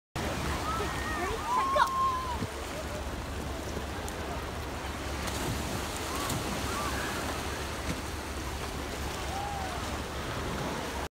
2020 Feb Hawaii Jumping into the ocean
At Waikiki beach, they have this jetty-type thing that the kids could run and jump off of into the water.